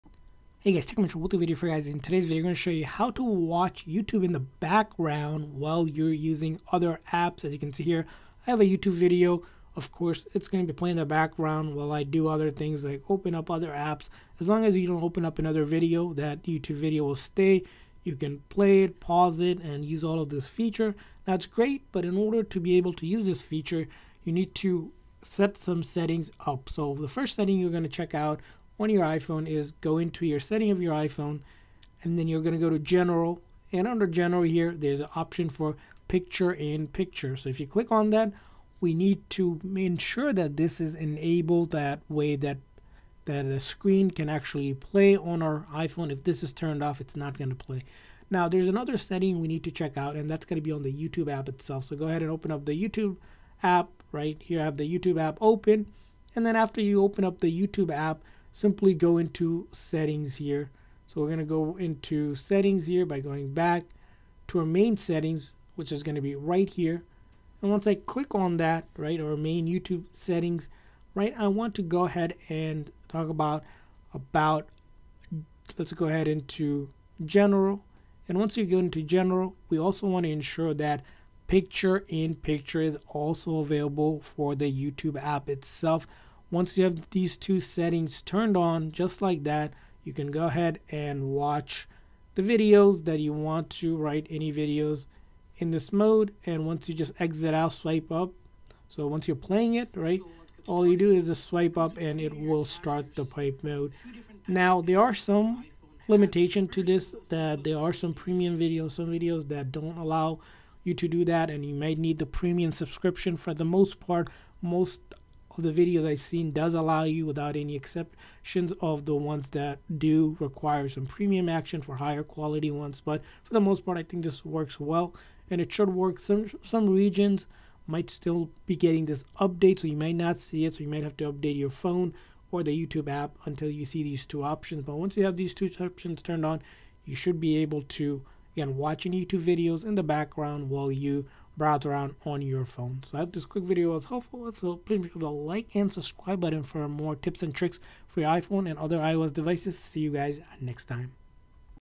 How to Play YouTube Videos in Background While Using Your iPhone
Want to play Youtube videos on the background while still using your iPhone to do other things? In this tutorial, we show you 2 settings you need to enable and turn on for YouTube to play in the background as picture in picture.